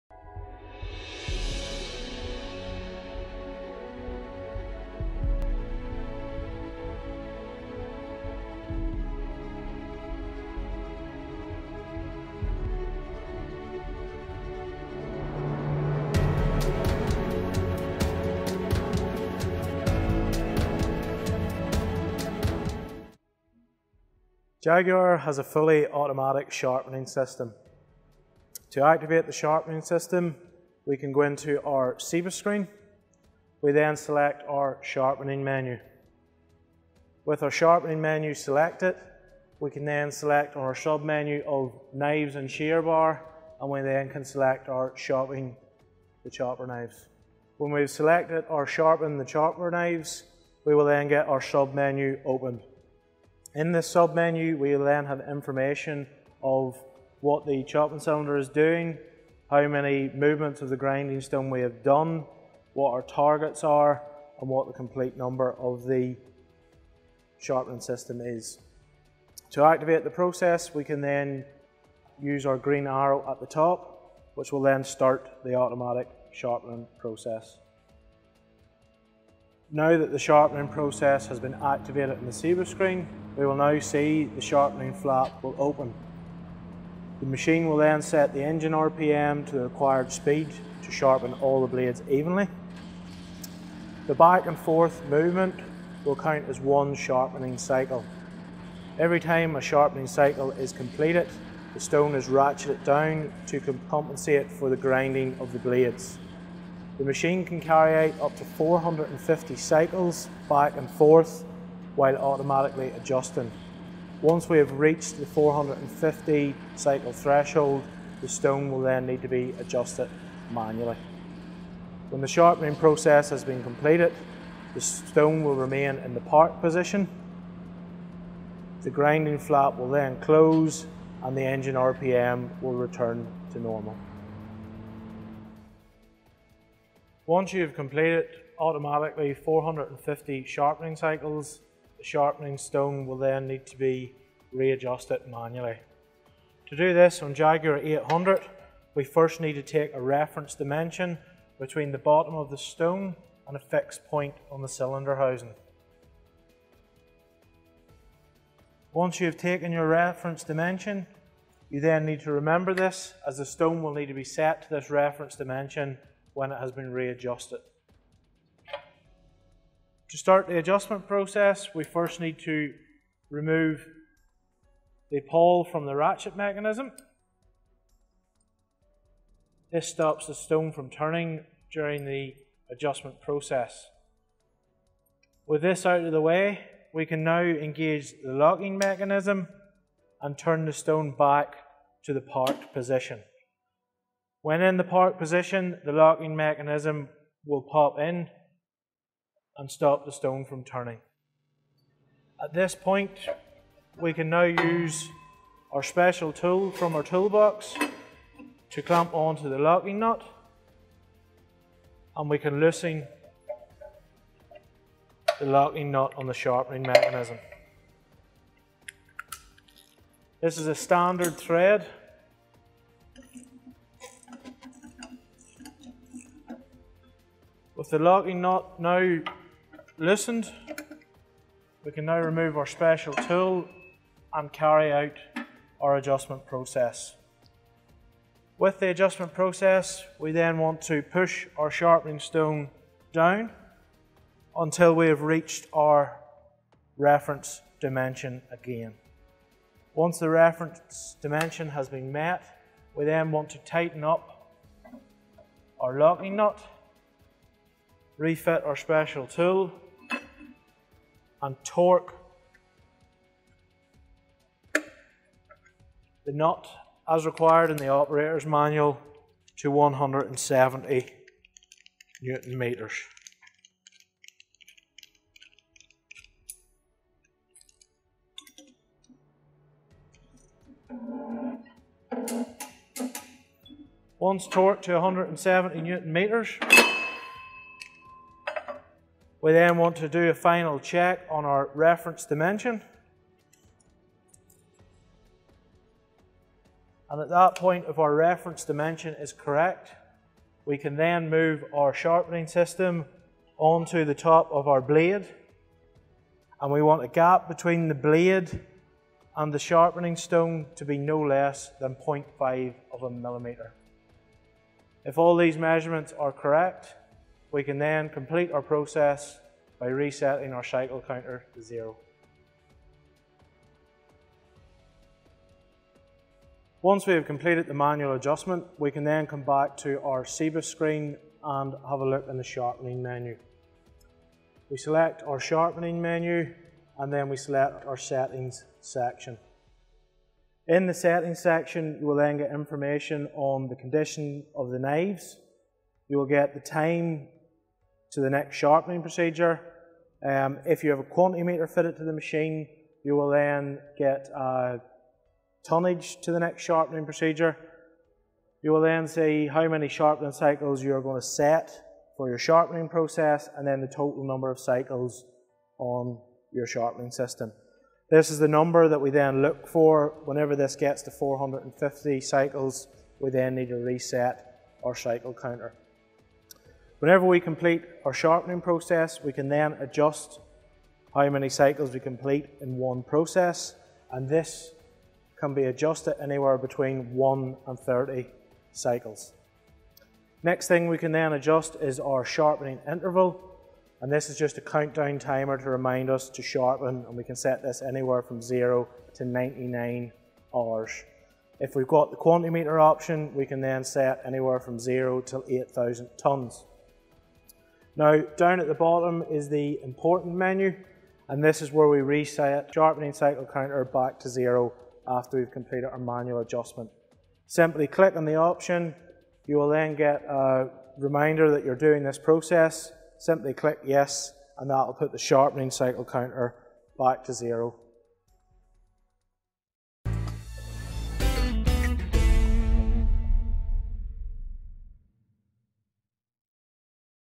JAGUAR Operator Tips - JAGUAR 800: Sharpening System Operation and Adjustment
This video provides an introduction on how to operate and adjust the sharpening system on JAGUAR 800 machines. Enjoy!